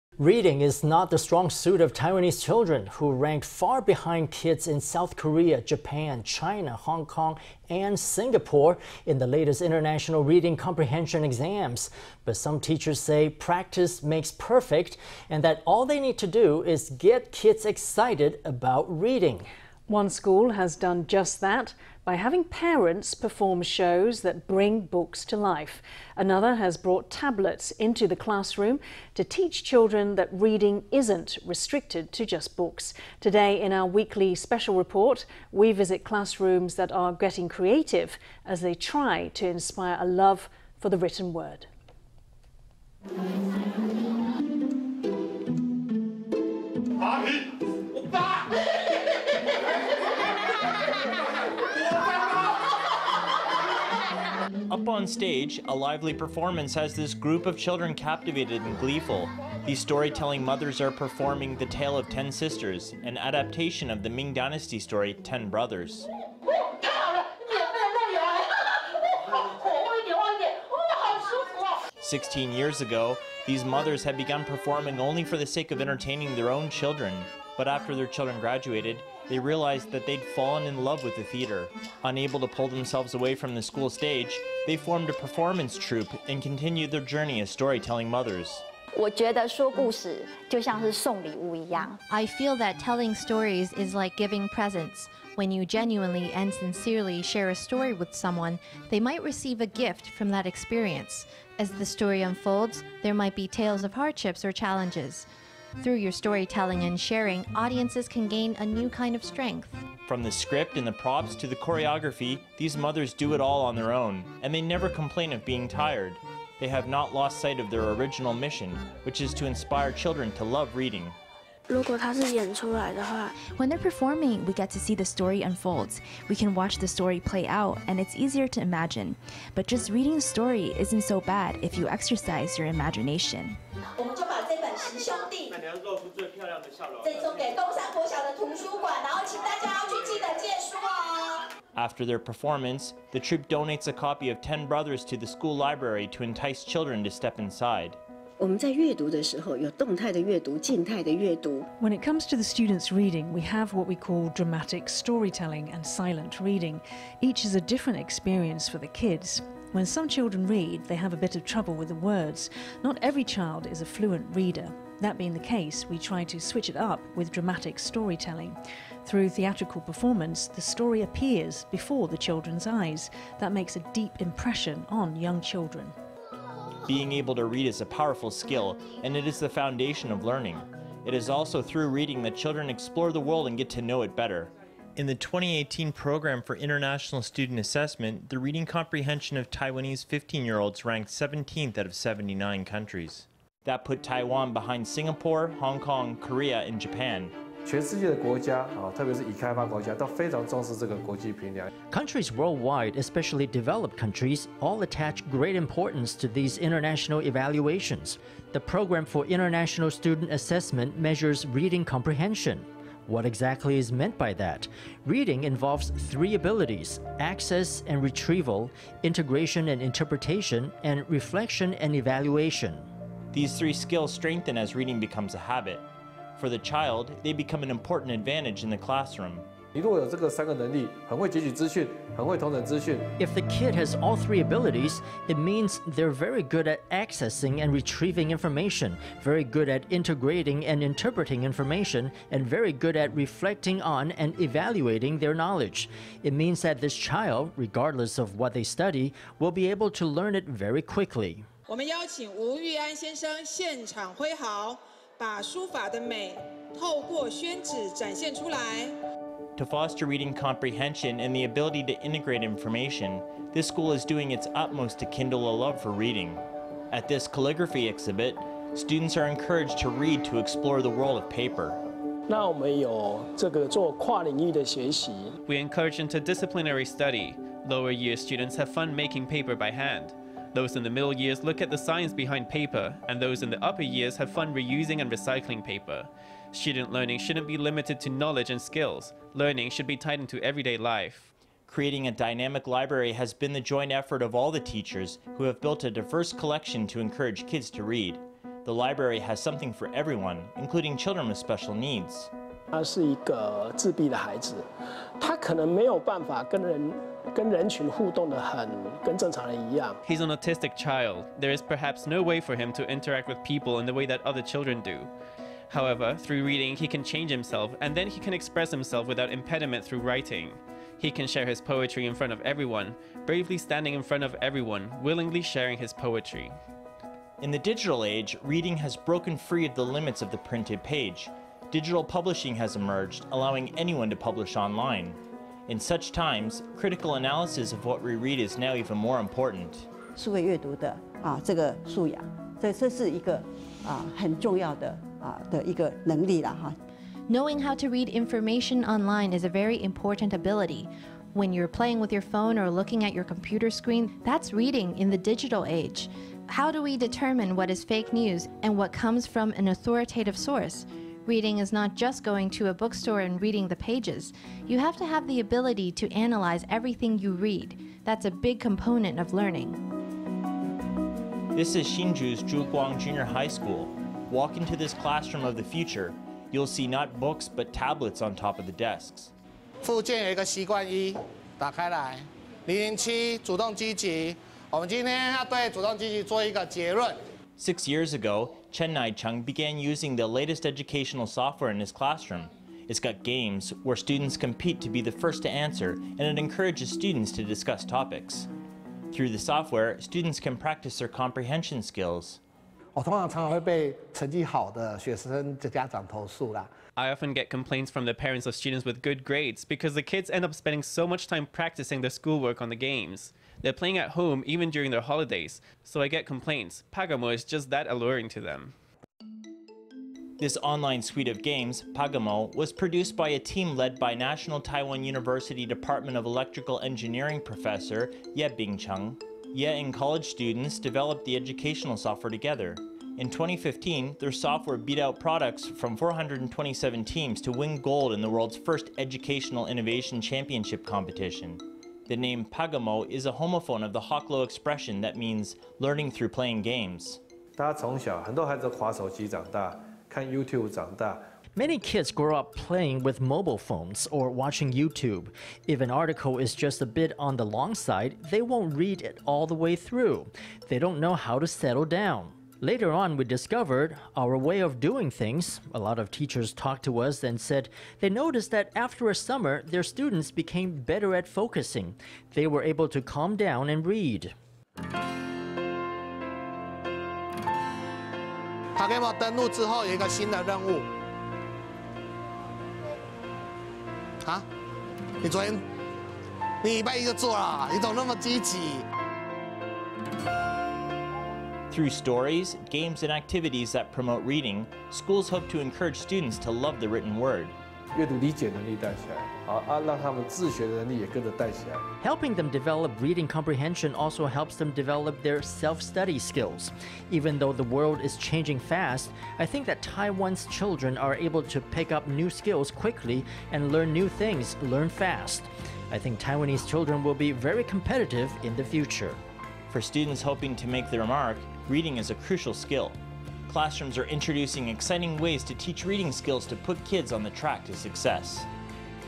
Schools testing new ways to inspire zest for reading
Reading is not the strong suit of Taiwanese children, who ranked far behind kids in South Korea, Japan, China, Hong Kong, and Singapore in the latest international reading comprehension exams. But some teachers say practice makes perfect, and that all they need to do is get kids excited about reading. One school has done just that, by having parents perform shows that bring books to life. Another has brought tablets into the classroom, to teach kids that reading isn’t restricted to books. Today in our weekly special report, we visit classrooms that are getting creative as they try to inspire a love for the written word. Up onstage, a lively performance has this group of children captivated and gleeful. These storytelling mothers are performing “The Tale of Ten Sisters” – an adaptation of the Ming Dynasty story “Ten Brothers.”Sixteen years ago, these mothers had begun performing only for the sake of entertaining their own children. But after their children graduated, they realized that they’d fallen in love with theater. Unable to pull themselves away from the school stage, they formed a performance troupe and continued their journey as storytelling mothers. Wang Chien-chienTroupe leaderI feel that telling stories is like giving presents. When you genuinely and sincerely share a story with someone, they receive a gift from that experience. As the story unfolds, there might be tales of hardships or challenges. Through your storytelling and sharing, audiences can gain a new kind of strength.From the script and the props to the choreography, these mothers do it all on their own, and they never complain of being tired. They have not lost sight of their original mission, which is to inspire children to love reading.Chen Pei-niElementary school studentWhen they’re performing, we get to see the story unfold. We can watch the story play out and it’s easier to imagine. But just reading the story isn’t so bad if you exercise your imagination.After their performance, the troupe donates a copy of “Ten Brothers” to the school library to entice children to step inside.Lin Hsiu-yunSchool principalWhen it comes to the students reading, we have what we call dramatic storytelling and silent reading. Each is a different experience for the kids. When some children read, they have a bit of trouble with the words. Not every child is a fluent reader. That being the case, we try to switch it up with dramatic storytelling. Through theatrical performance, the story appears before the children’s eyes. That makes a deep impression on young children. Being able to read is a powerful skill, and it is the foundation of learning. It is also through reading that children explore the world and get to know it better. In the 2018 Program for International Student Assessment, the reading comprehension of Taiwanese 15-year-olds ranked 17th out of 72 countries. That put Taiwan behind Singapore, Hong Kong, Korea and Japan.Yeh Ping-chengNational Taiwan University professorCountries worldwide – especially developed countries – all attach great importance to these international evaluations. The Program for International Student Assessment measures reading comprehension. What exactly is meant by that? Reading involves three abilities: access and retrieval, integration and interpretation, and reflection and evaluation.These three skills strengthen as reading becomes a habit. For the child, they become an important advantage in the classroom.Yeh Ping-chengNTU professorIf the kid has all three abilities, it means they’re very good at accessing and retrieving information, very good at integrating and interpreting information, and very good at reflecting on and evaluating their knowledge. It means that this child, regardless of what they study, will be able to learn it very quickly. To foster reading comprehension and the ability to integrate information, this school is doing its utmost to kindle a love for reading. At this calligraphy exhibit, students are encouraged to read to explore the world of paper. Ho Yuan-hengSchool principalWe encourage interdisciplinary study. Lower-year students have fun making paper by hand. Those in the middle years look at the science behind paper, and those in the upper years have fun reusing and recycling paper. Student learning shouldn’t be limited to knowledge and skills. Learning should be tied into everyday life.Creating a dynamic library has been the joint effort of all the teachers, who have built a diverse collection to encourage kids to read. The library has something for everyone, including children with special needs.Ho Yuan-hengSchool principalHe is an autistic child. There is perhaps no way for him to interact with people in the way that other children do. However, through reading, he can change himself, and then he can express himself without impediment through writing. He can share his poetry in front of everyo...